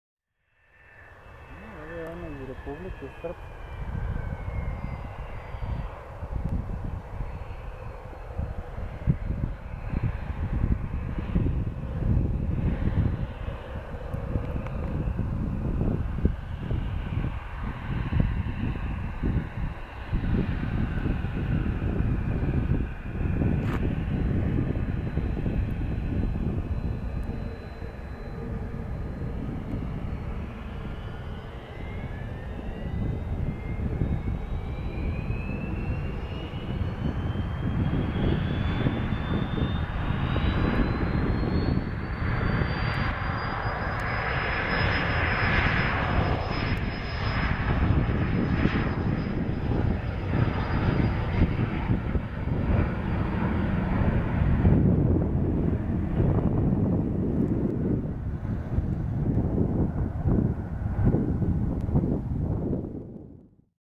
[Full HD]Government Of Bosnia Cessna CitationJet CJ1 Takeoff From Belgrade Airport
Belgrade(BEG)➡?
Airline: Government of Bosnia
Aircraft: Cessna CitationJet CJ1+
Registration: E7-SMS
Built In 2008.
Delivered in 2009.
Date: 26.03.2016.
Time: 13:48pm

COPYRIGHT: Plane Spotting TV